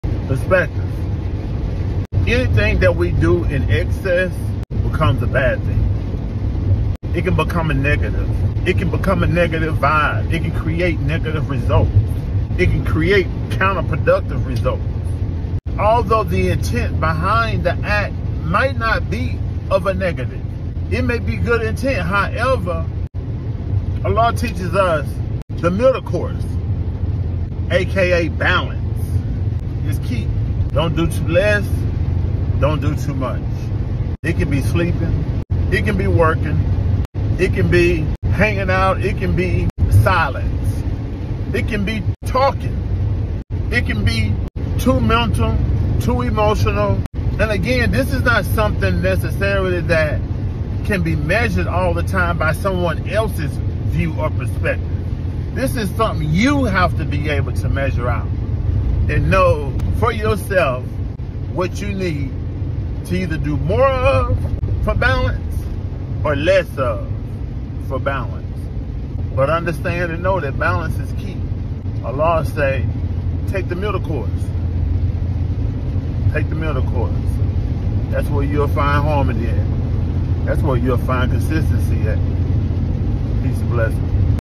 Balance is Key!
As the saying went, “ You’ve tried it your way, now try God.” And allow God’s way, to be sufficient for you in EVERY aspect of your Life! Not just in some areas of it.